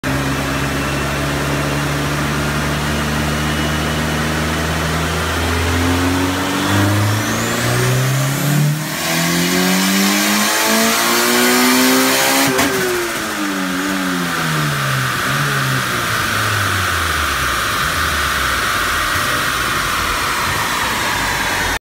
Rocky's beautiful B20V boosted Eg hatch tuned by Eddie of Genesis Automotive
This bone stock B20V on 8psi and pump gas was tuned by Eddie of Genesis Automotive and put out 231hp / 183tq (car was detuned for customers son). Very quick and fun ride!